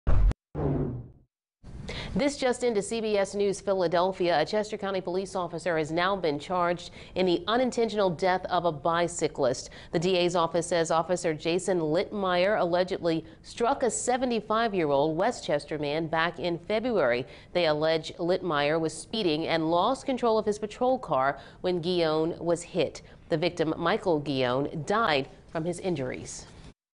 Chester Co. police officer charged in unintentional death of bicyclist
Jason Listmeier allegedly struck and killed 75-year-old Michael Ghione, of West Chester, on Feb. 19.